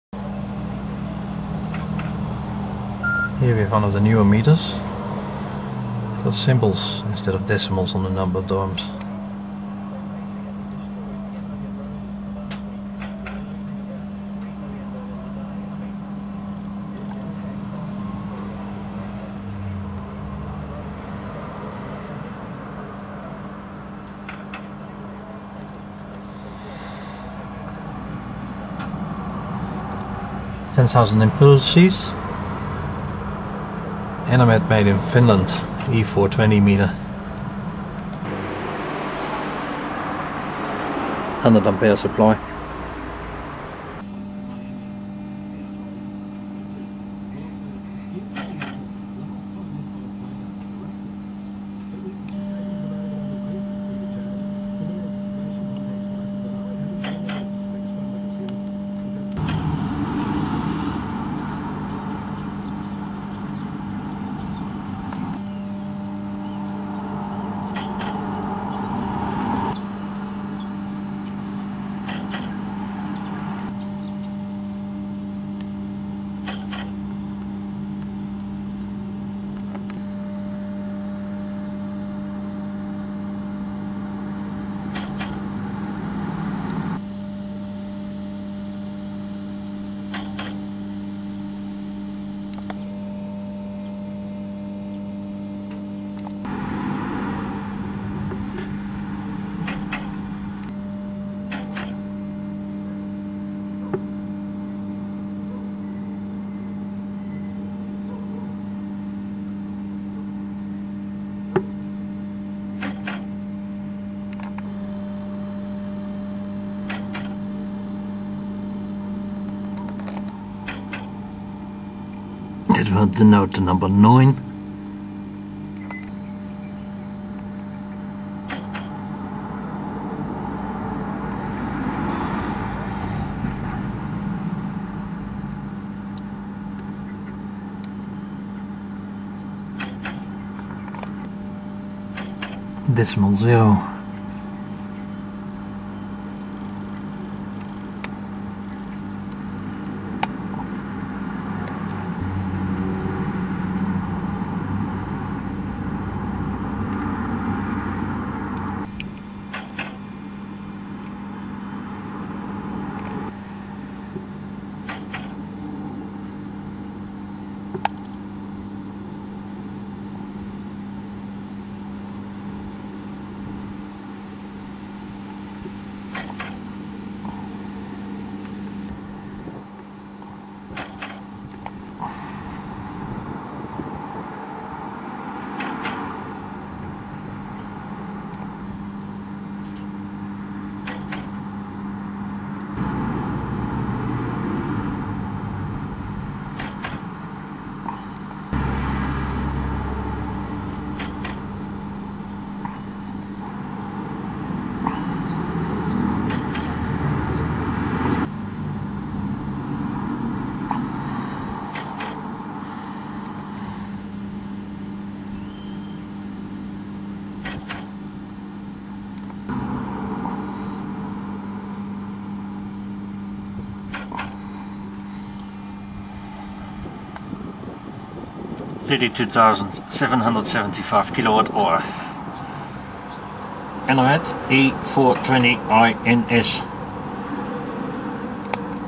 Enermet E420i kWh meter
It is interesting to see how different hour glass type patterns are used to display the decimals on a modern kilowatt hour meter instead of the normal numbers.

This meter is an Enermet 3 phase 100 Ampère kWh meter.

The red impulsing led speed was not correctly picked up by the camera as it was pulsing faster than the 24 FPS of the video.

decimal 0 at 0:56 and 2:35
decimal 1 at 2:58
decimal 2 at 1:03 and 3:39
decimal 3 at 1:12 and 3:44
decimal 4 at 1:19
decimal 5 at 1:23
decimal 6 at 1:32
decimal 7 at 1:55
decimal 8 at 2:00
decimal 9 at 0:47 and 2:04